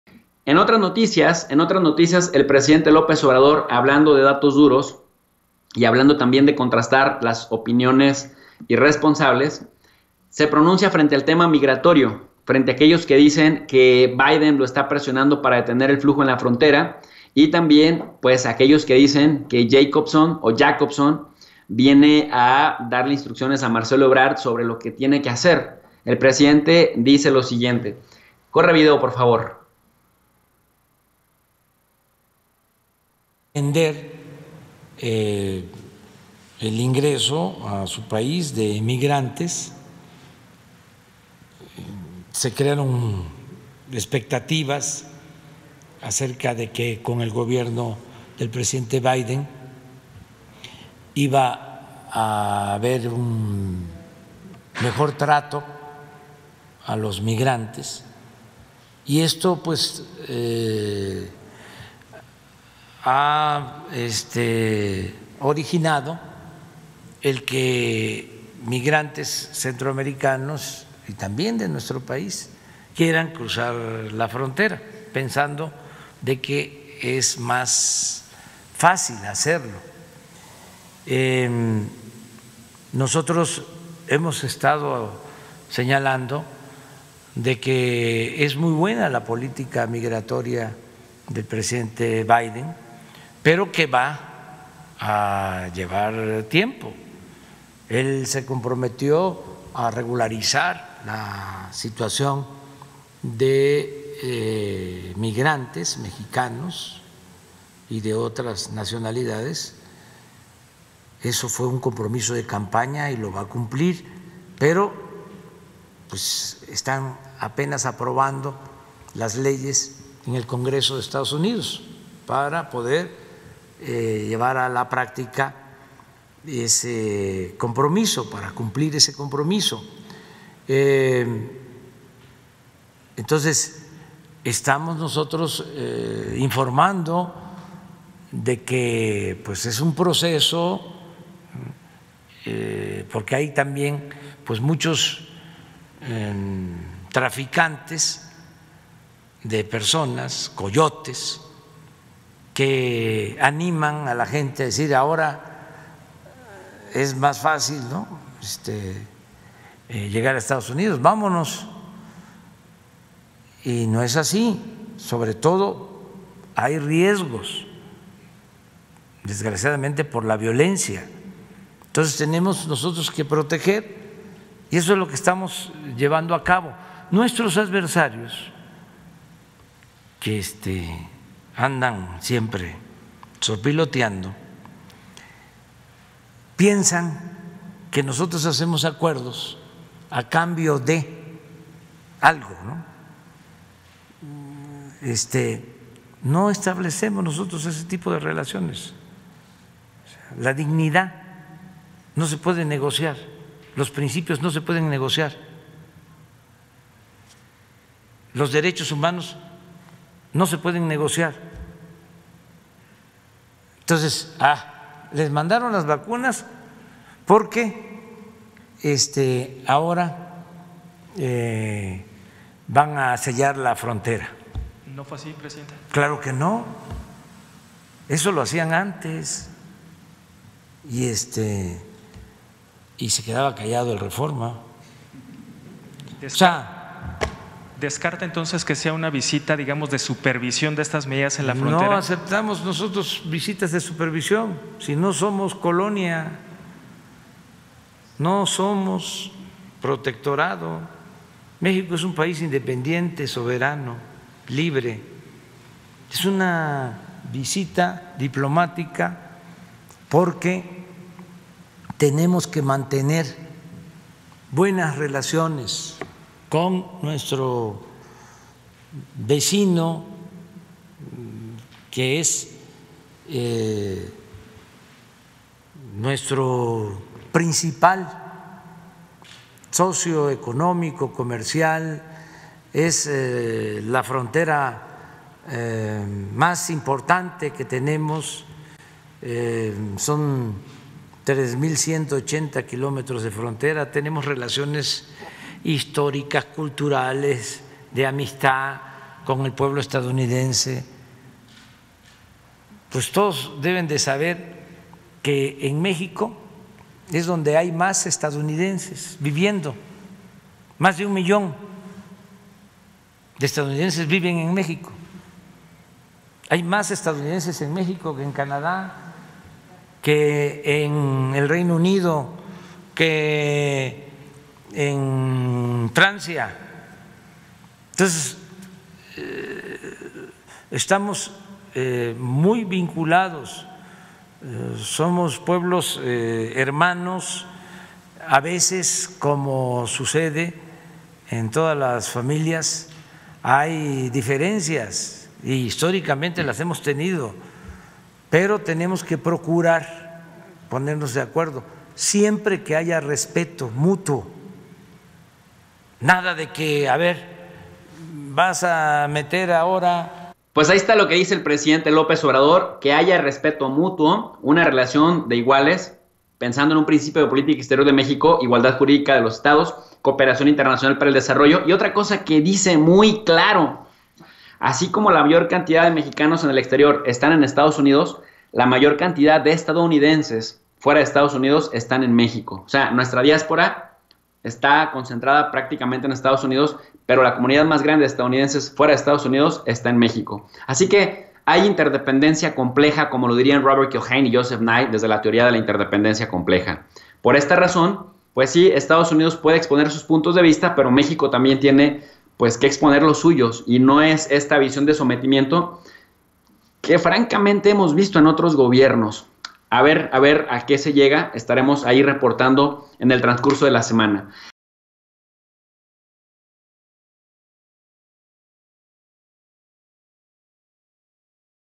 AMLO SE PRONUNCIA SOBRE CRISIS MIGRATORIA EN LA FRONTERA: ENCUENTRO EBRARD-JACOBSON
El presidente de México, Andrés Manuel López Obrador, señaló que la charla sostenida entre las delegaciones mexicana y estadounidense, encabezadas por el canciller Ebrard y Roberta Jacobson, se circunscriben en el marco de una cooperación conjunta para el desarrollo y para enfrentar desafíos comunes. Aquí te presentamos el discurso completo.